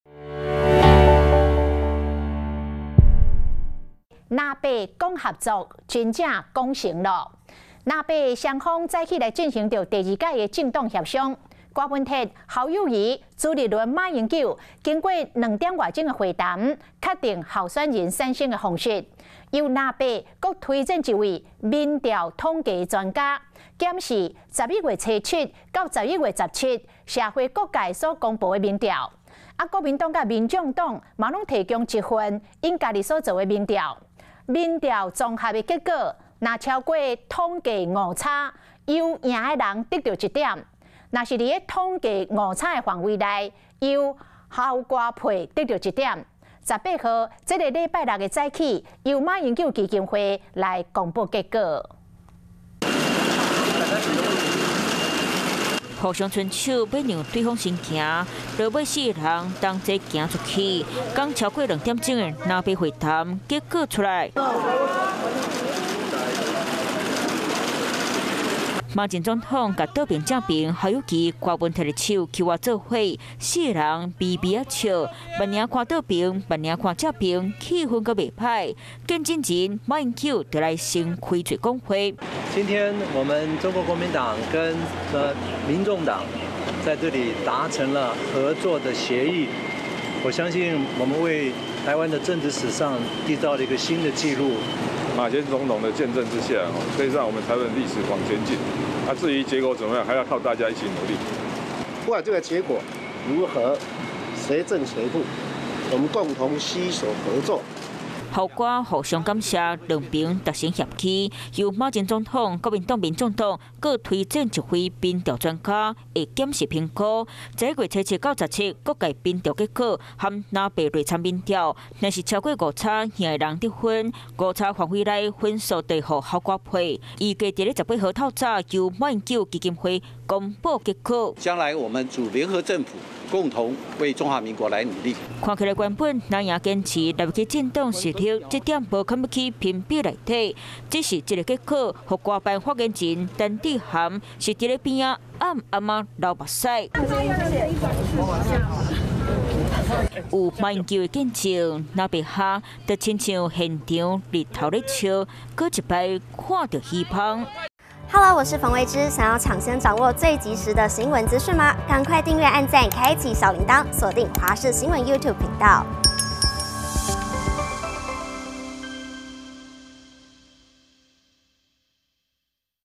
藍白整合！侯.柯依「現有民調」勝負 18日公布結果｜華視台語新聞 2023.11.15
2024總統大選，在野的藍白合，在柯文哲、侯友宜、朱立倫、馬英九，超過兩小時的「四方會商」後，決定候選人的產生方式，依照現有民調。將由馬英九、國民黨、民眾黨三方，共推民調專家，檢視11月7日到17日，各界的民調數字，以及藍白內參民調，如果超過誤差「勝者得分」，誤差範圍內分數給「侯柯配」，預計在18日上午，由馬英九基金會公布結果。